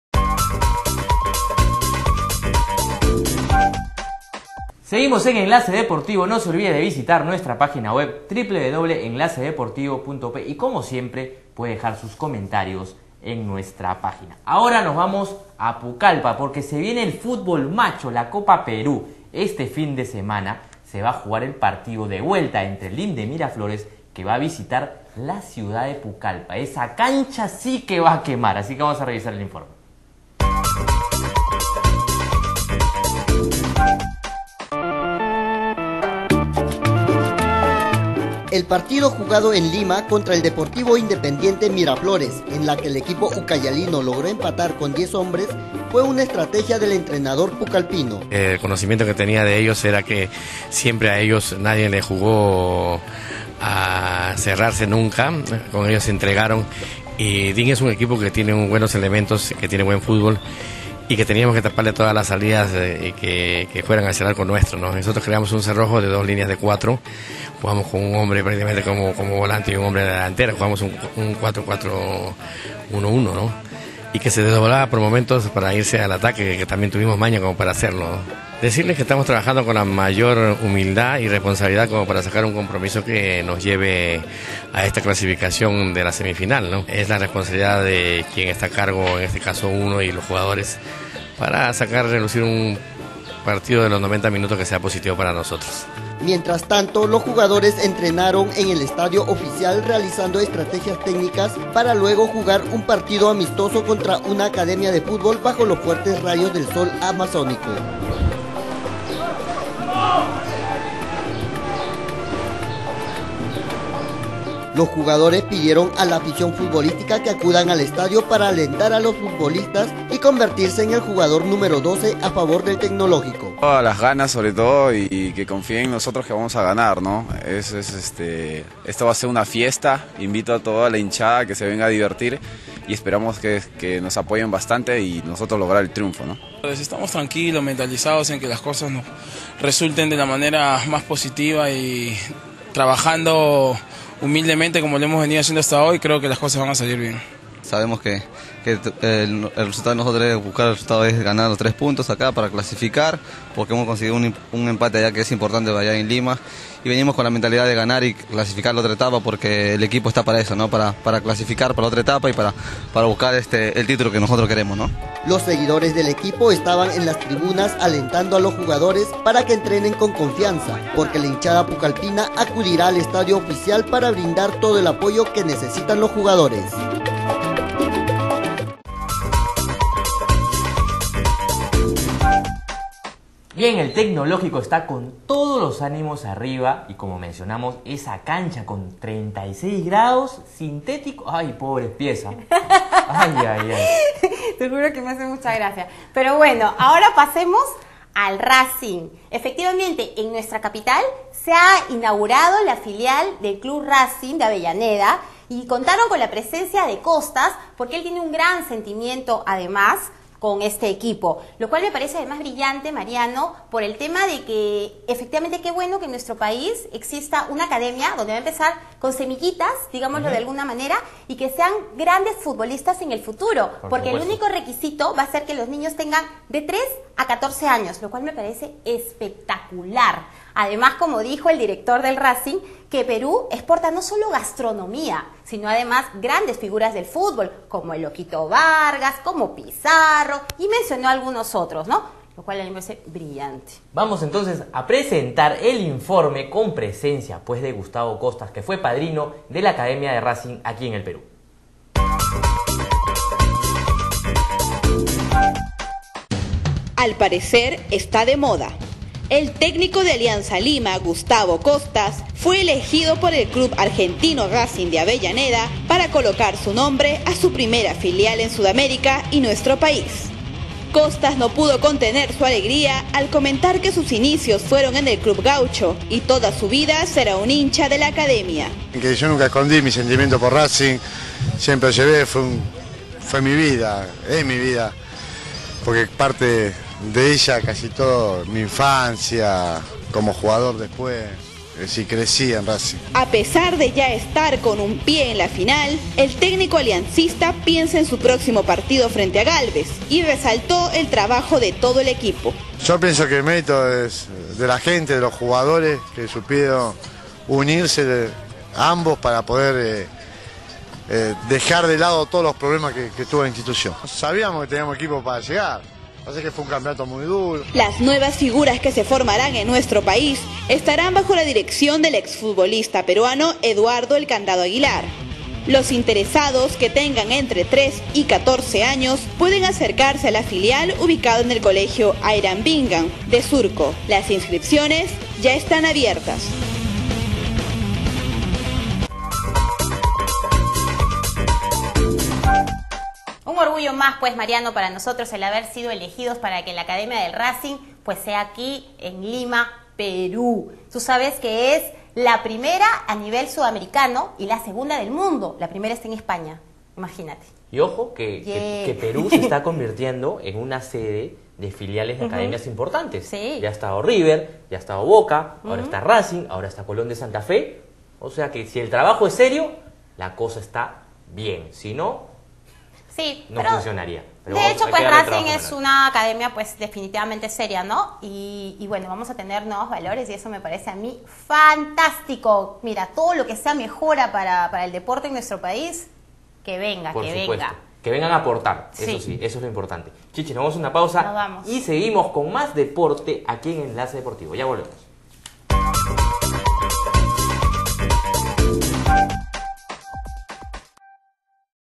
Gustavo Costas, El Padrino
Copa Perú: Con el Calor a su favor, Tecnológico listo para recibir al DIM en el sintético de Pucallpa.
Gustavo Costas apadrinó Academia de Racing en el Perú que ya inició actividades.